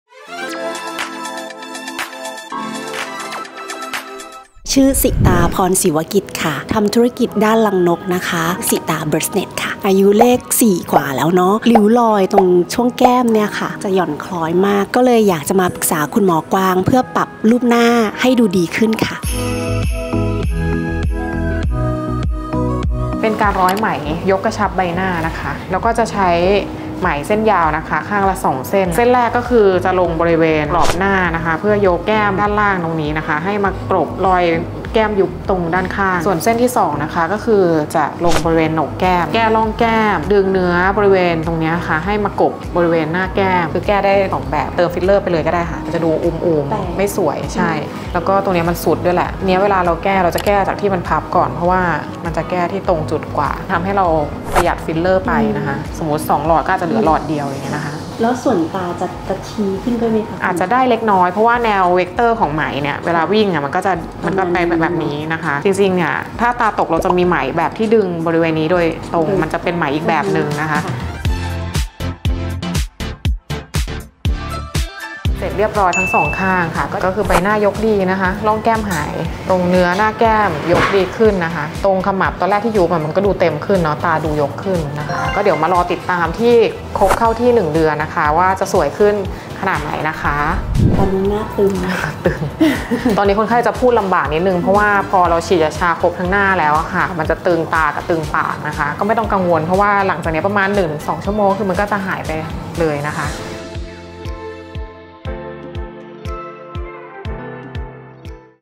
⚠️จบปัญหาคนอายุ 40+ รีวิวยกร่องแก้มด้วยการร้อยไหม ไม่ต้องพึ่งฟิลเลอร์✨
เมื่ออายุมากขึ้นปัญหาเรื่องริ้วรอย ความหย่อนคล้อย ขมับตอบและอีก
หลายปัญหาบนใบหน้าก็เริ่มชัดขึ้น การที่จะหาหัตถการมาดูแล ฟื้นฟู
หรือแก้ปัญหาทั้งหมดก็เป็นไปได้ยาก 😋

เคสนี้ อาจารย์หมอกวางโชว์การร้อยไหมแก้ปัญหาบนใบหน้าให้เจ้าแม่รังนกอย่าง คุณสิตา🐦 หลังทำจะเห็นได้ว่าใบหน้ามีความกระชับ ขมับดูเต็มแถมตาดูยกขึ้นและร่องแก้มหายไป อย่างเห็นได้ชัด  😍

การร้อยไหมนั้น หากร้อยด้วยเทคนิคที่ถูกต้องก็จะเกิดเป็นเส้นใยอิลาสตินช่วยประคองผิว และสามารถดึงแก้มที่หย่อนขึ้นได้ทันทีแถมยังช่วยกระตุ้นคอลลาเจนใต้ชั้นผิวได้อีกด้วย หากใครมีปัญหาเรื่องความหย่อนคล้อย ร่องลึกหรือมีความกังวลกับปัญหาแห่งวัยบนใบหน้า📌

สามารถเข้ามาปรึกษาอาจารย์แพทย์ที่ วลีรัตน์ คลินิก เพื่อวิเคราะห์ปัญหาและวางแผนการรักษาให้ได้ผลลัพธ์ตรงตามความต้องการและแก้จุดบกพร่องบนใบหน้าได้อย่างมีประสิทธิภาพ👍👩‍⚕️🧑‍⚕️